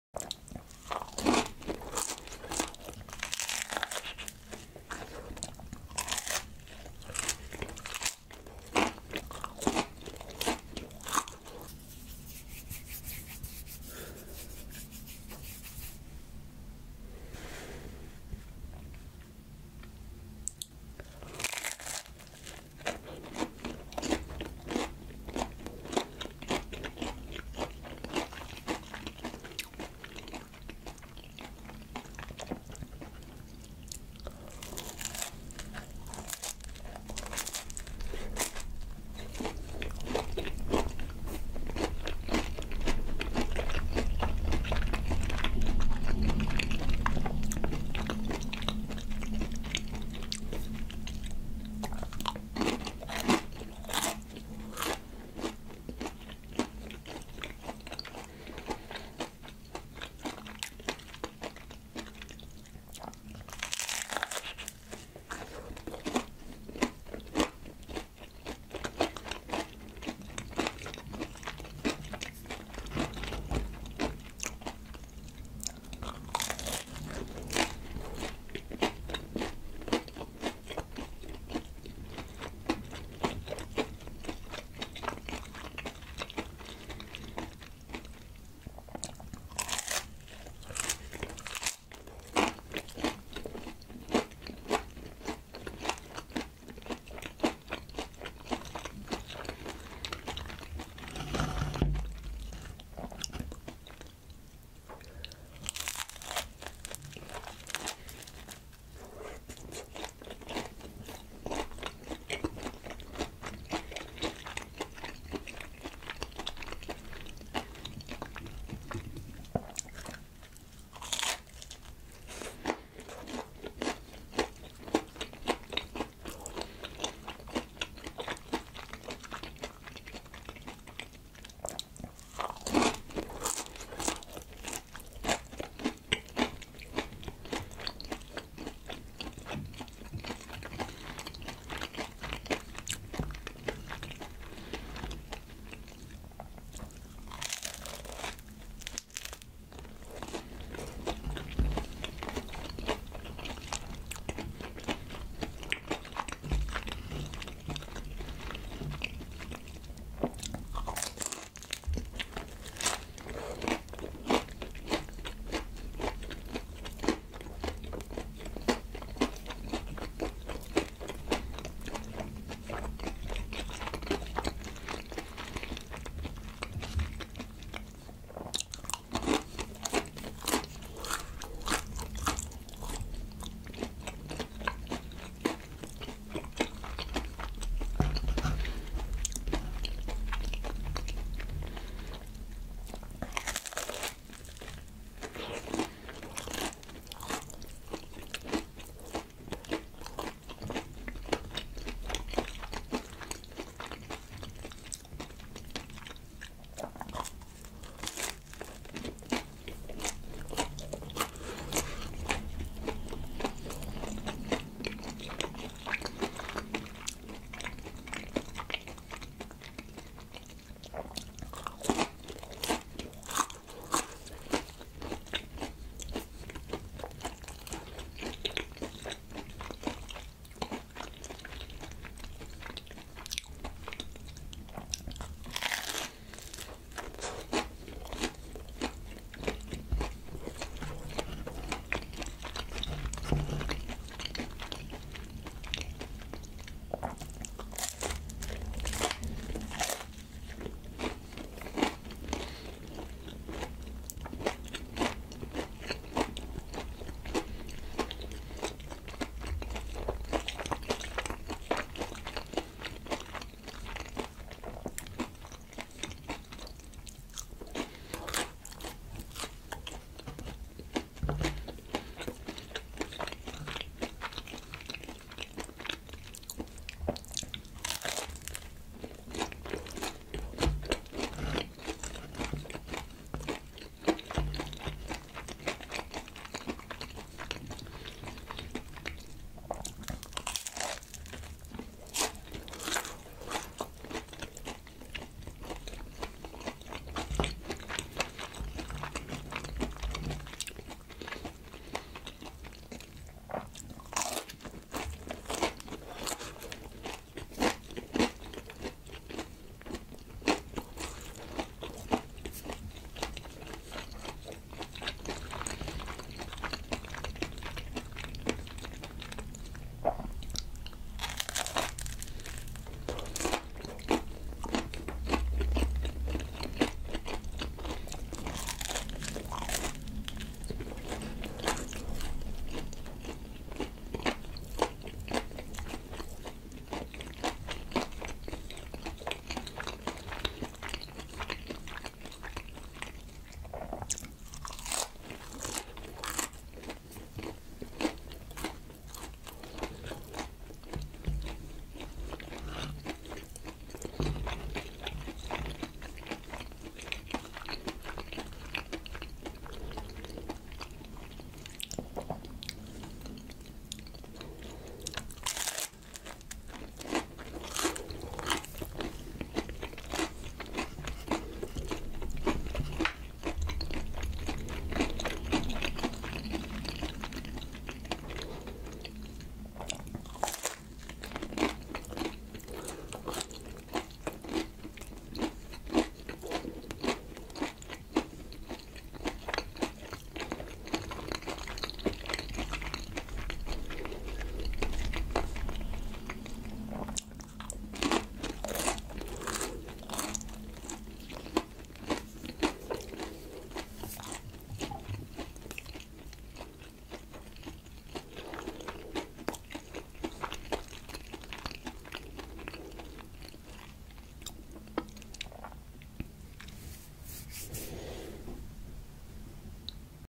ASMR PICCOLINIS MINI PIZZA MUKBANG | No Talking Eating Sounds Chill Down ASMR
~ Chill Down Asmr

This is an ASMR EATING VIDEO. To get tingles, PLEASE WEAR HEADPHONES. If you don't like eating sounds, stop watching.

▬ My Gear ▬▬▬▬▬▬▬▬▬▬▬▬

Cut Board